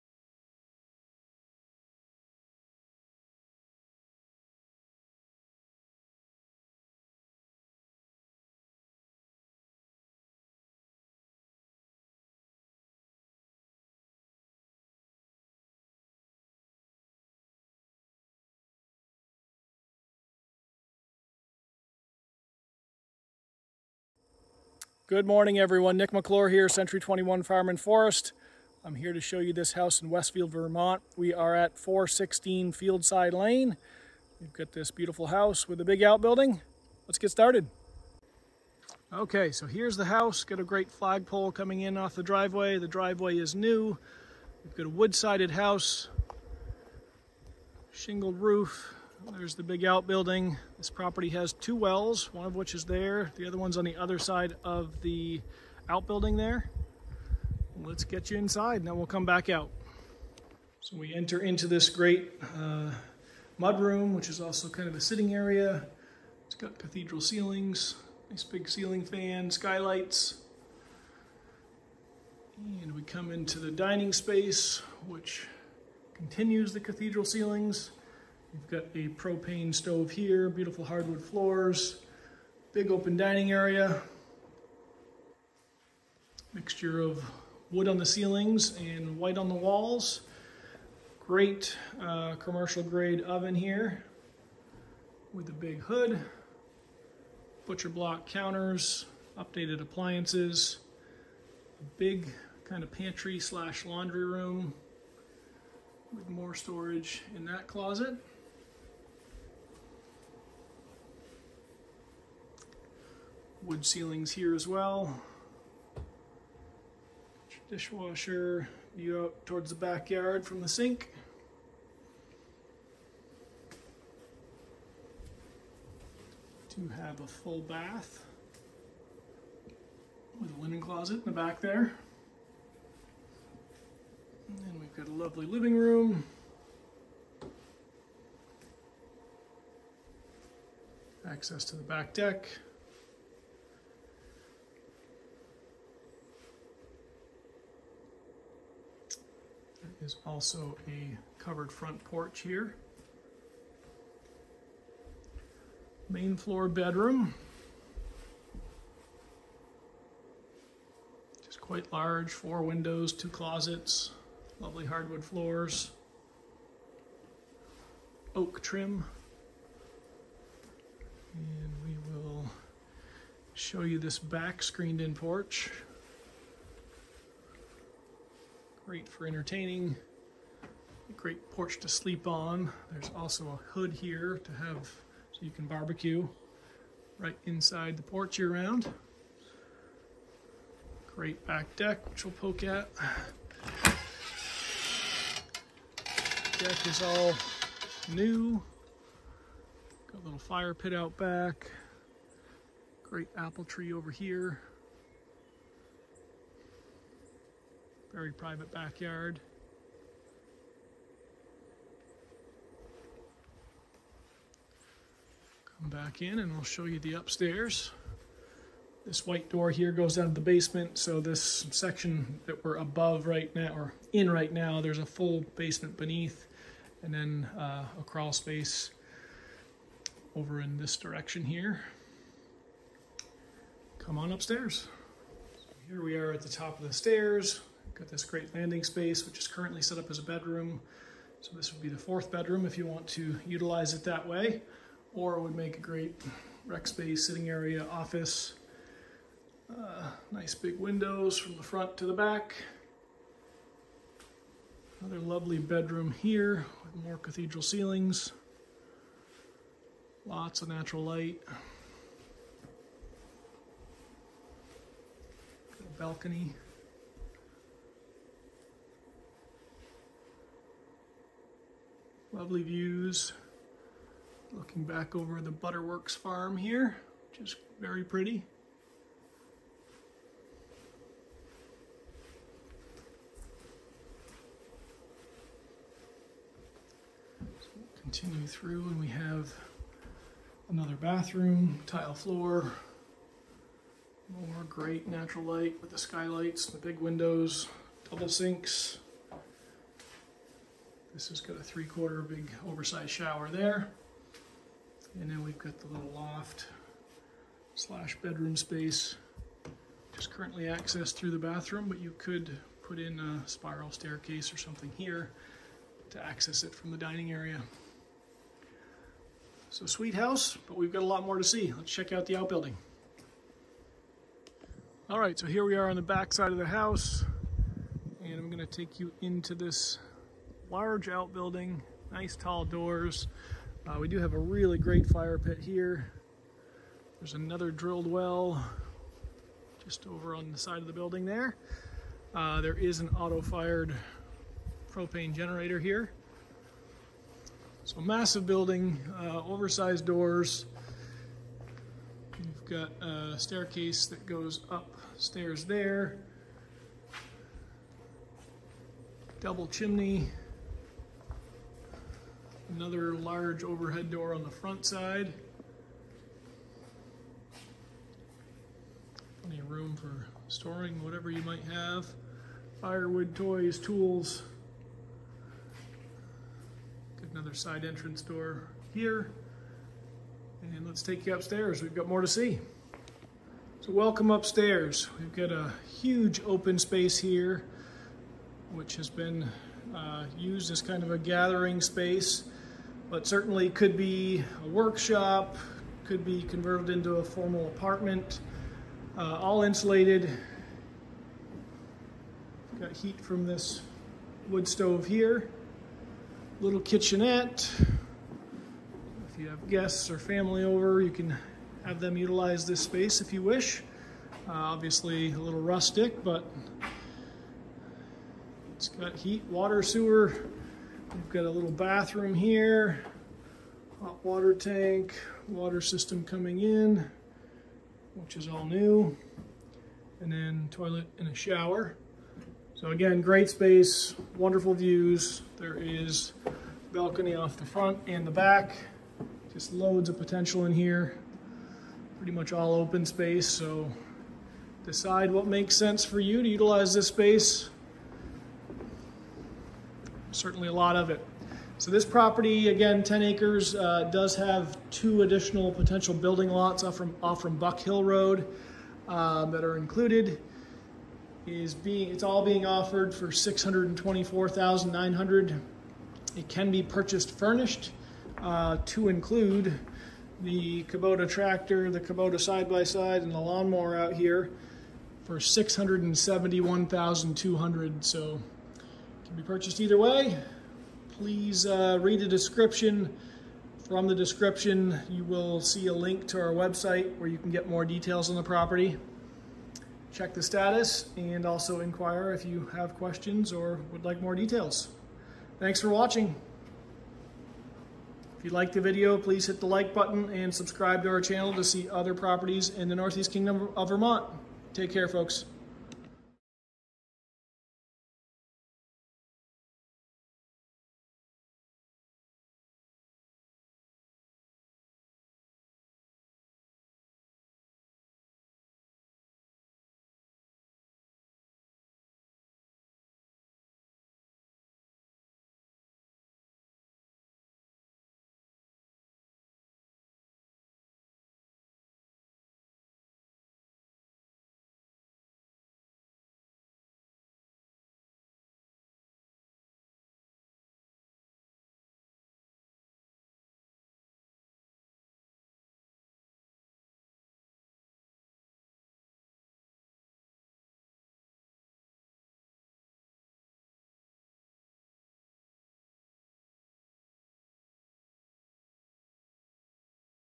The Soaring Pines Homestead - Home For Sale - 216 Field Stone Lane, Westfield, VT
PRICE REDUCED TO $599,900!   Welcome to the Soaring Pines Homestead or Retreat. Here we have a 3-4 bedroom 2 bath home loaded with character, plus a large shop building with an upstairs that could be an apartment, an airbnb, a work shop, studio, game room, etc. Furthermore, we also have 10 acres of land with a small private pond & field. The land has frontage on Fieldside Lane which is where the house is but also frontage on Buck Hill. The Buck Hill frontage has been engineered & is getting permitted for two additional 2 bedroom homes or cabins so you have potential for development, cabins, guest homes, or to sell off some of the land. Inside the home you will find a large entry / sitting area which opens into a dining room with a gas stove, both with cathedral ceilings. The kitchen features updated appliances including a massive gas range. Continue into the living room where you can access the front covered porch or the back deck. A main bedroom, full bathroom, a laundry / pantry room & a large screened in porch set up for grilling wrap up the main level. Head upstairs and the center room is currently set up as a bedroom but could be a sitting area / office or den, a gracious bedroom with a balcony, a 3/4 bath & a loft bedroom. Contain all of the hobbies, toys & tools in the huge outbuilding with oversized double doors. This property is only about 12 miles to Jay Peak Resort, 17 miles to Newport & 26 to Morrisville.

Listed by Nicholas Maclure at Century 21 Farm & Forest for $599,900.